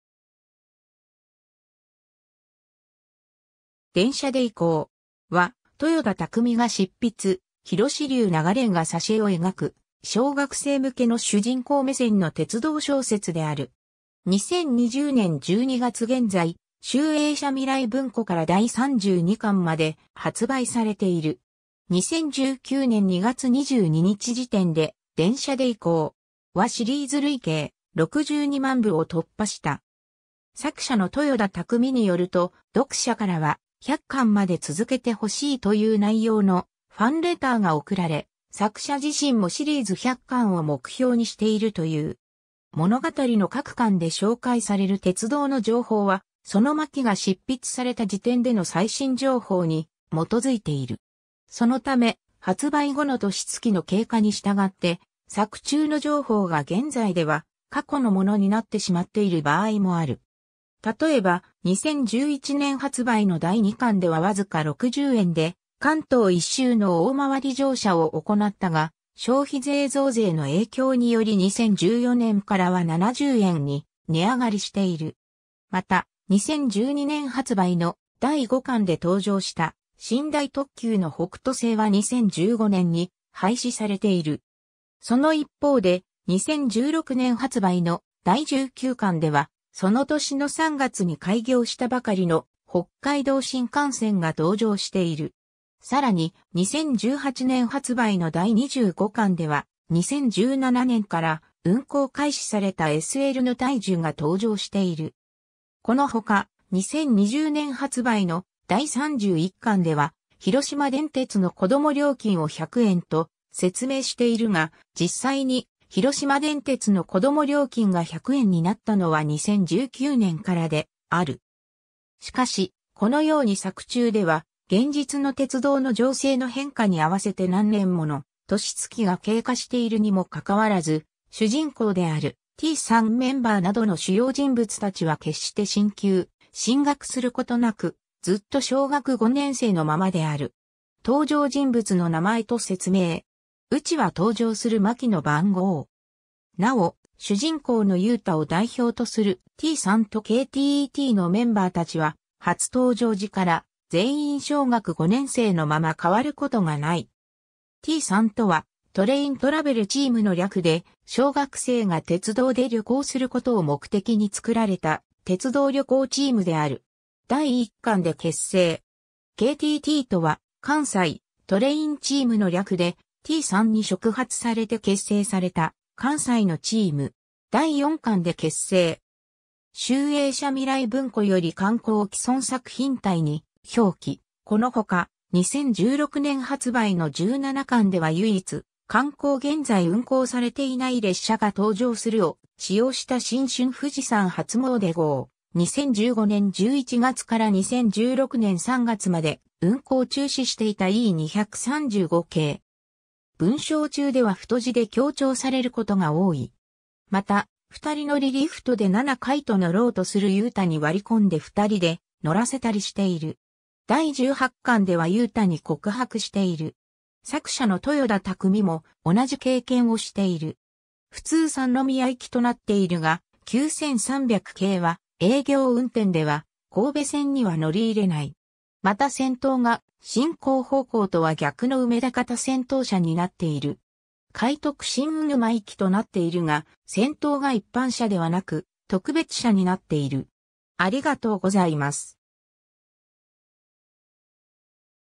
電車で行こう!
『電車で行こう!』（でんしゃでいこう）は、豊田巧が執筆、裕龍ながれが挿絵を描く小学生向けの主人公目線の鉄道小説である。
 2020年12月現在、集英社みらい文庫から第32巻（スペシャル版を除く）まで発売されている。
2019年2月22日時点で『電車で行こう!』はシリーズ累計『62万部』を突破した。
 作者の豊田巧によると、読者からは「100巻まで続けてほしい」という内容のファンレターが送られ、作者自身もシリーズ100巻を目標にしているという（第6巻、第13巻、第14巻のあとがきより）。
 物語の各巻で紹介される鉄道の情報は、その巻が執筆された時点での最新情報に基づいている。
そのため、発売後の年月の経過に従って、作中の情報が現在では過去のものになってしまっている場合もある。
例えば、2011年発売の第2巻ではわずか60円（子供料金）で関東一周の大回り乗車を行ったが、消費税増税の影響により2014年からは70円（子供料金）に値上がりしている。
また、2012年発売の第5巻で登場した寝台特急の北斗星は2015年に廃止されている。
その一方で、2016年発売の第19巻では、その年の3月に開業したばかりの北海道新幹線が登場している。
さらに、2018年発売の第25巻では、2017年から運行開始されたSL（蒸気機関車）の大樹が登場している。
この他、2020年発売の第31巻では、広島電鉄の子供料金を100円と説明しているが、実際に広島電鉄の子供料金が100円になったのは2019年からである。
しかし、このように作中では現実の鉄道の情勢の変化に合わせて何年もの年月が経過しているにも関わらず、主人公であるT3メンバーなどの主要人物たちは決して進級・進学することなく、ずっと小学5年生のままである。
 登場人物の名前と説明。
（ ）内は登場する巻の番号。
なお、主人公の雄太を代表とするT3とKTTのメンバー達は、初登場時から全員小学5年生のまま変わることがない。
 T3とはトレイン・トラベル・チーム (Train Travel Team) の略で、小学生が鉄道で旅行することを目的に作られた鉄道旅行チームである。
第1巻で結成。
 KTTとは関西・トレイン・チーム (Kansai Train Team) の略で、T3に触発されて結成された関西のチーム。
第4巻で結成。
 集英社みらい文庫より刊行 ^ 既存作品帯に表記 ^ この他、2016年発売の17巻では唯一刊行現在運行されていない列車が登場する（485系 彩（いろどり）を使用した新春富士山初詣号・2015年11月から2016年3月まで運行中止していた E235系）。
 ^ 文章中では太字で強調されることが多い。
また、二人乗りリフトで七海と乗ろうとする雄太に割り込んで二人で乗らせたりしている。
第18巻では雄太に告白している。
 ^ 作者の豊田巧も同じ経験をしている（第1巻 あとがき）。
 ^ 普通 三宮行きとなっているが、9300系は営業運転では神戸線には乗り入れない。
また先頭が進行方向とは逆の梅田方先頭車になっている。
 ^ 快特 新鵜沼行きとなっているが、先頭が一般車（6号車）ではなく特別車（1号車）になっている。